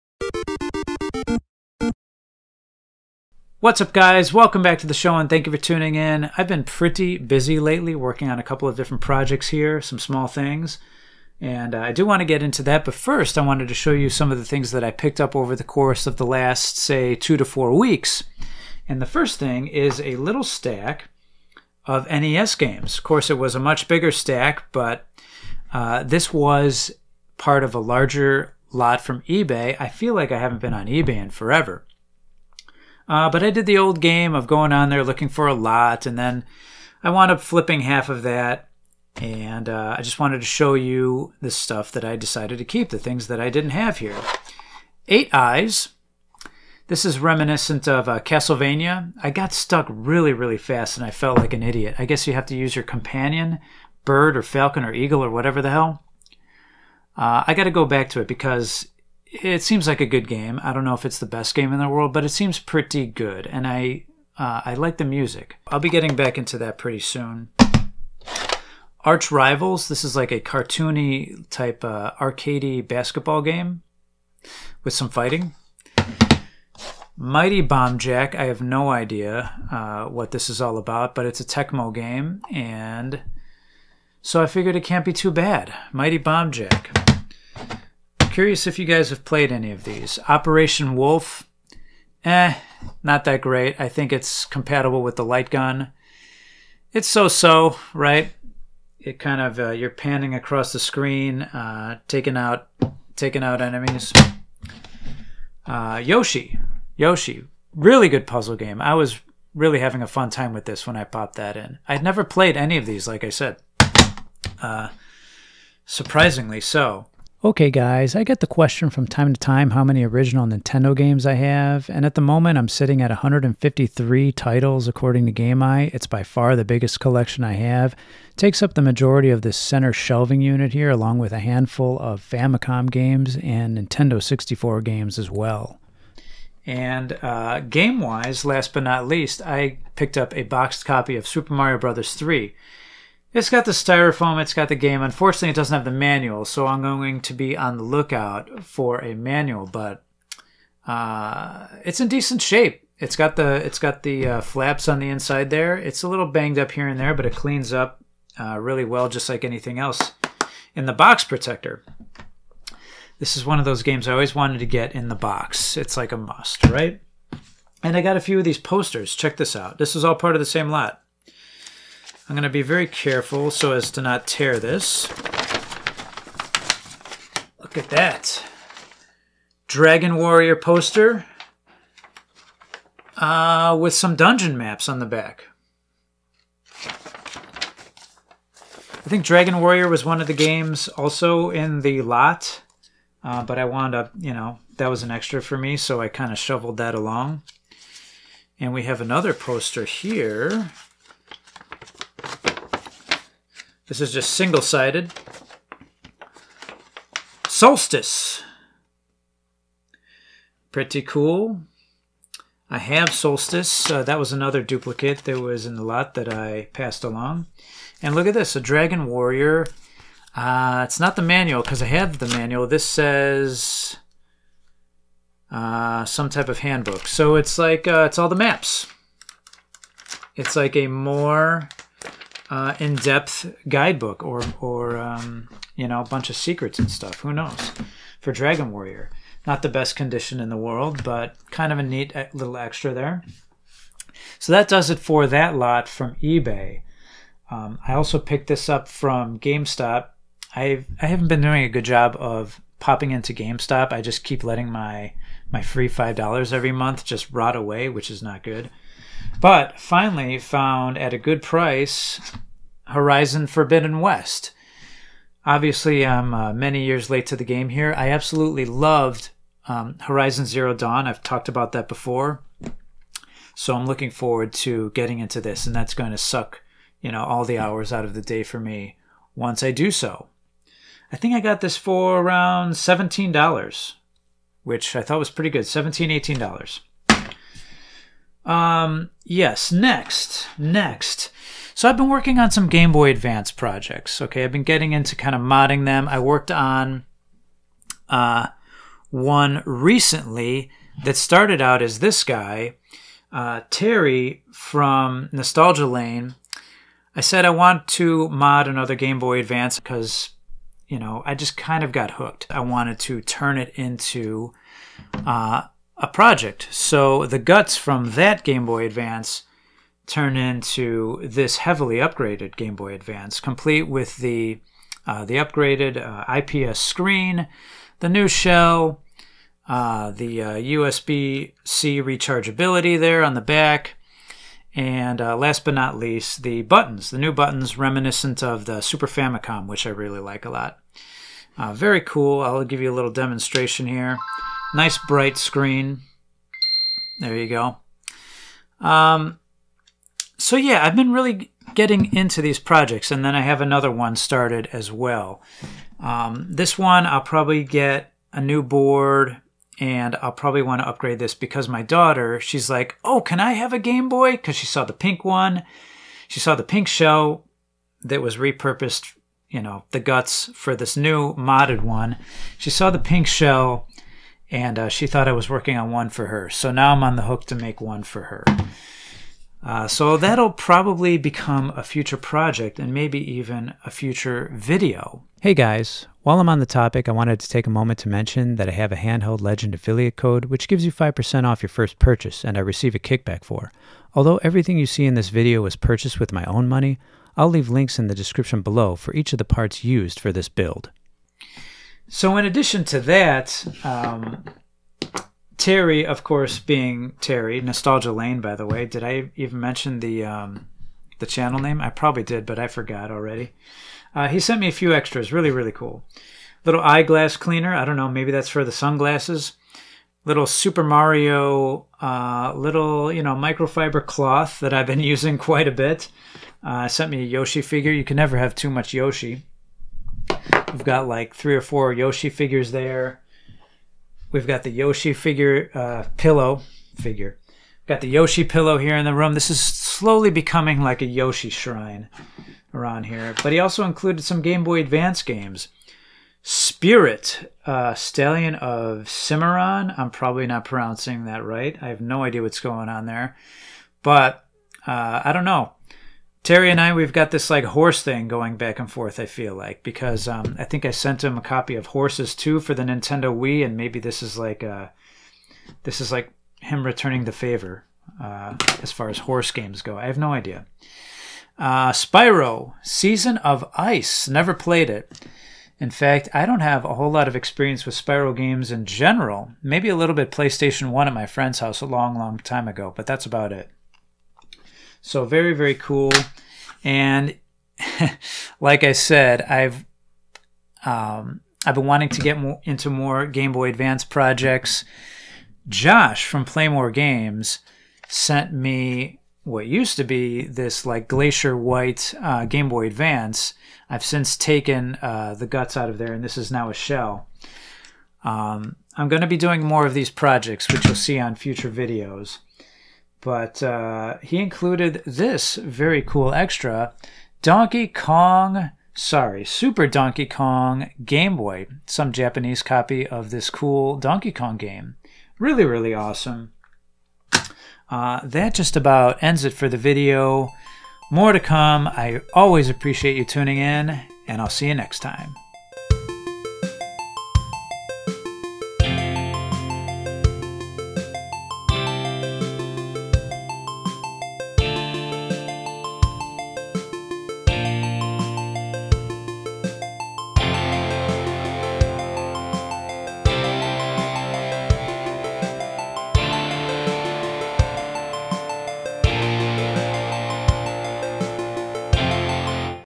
RETRO GAME PICKUPS AND NEW PROJECTS -Dad Racer
Today we talk about recent retro game pickups and some handheld console projects.

*Links to parts used for the Modded Game Boy Advance in this video:

Thank you @nostalgialane2684 and @playmooregames371 for sending the awesome stuff my way!!!

Timestamps:
00:00 Retro Games & Posters
04:07 Modern Game Pickup
04:50 Game Boy Advance Projects
06:45 Extra Stuff from Nostalgia Lane!
08:15 GBA from playmooregames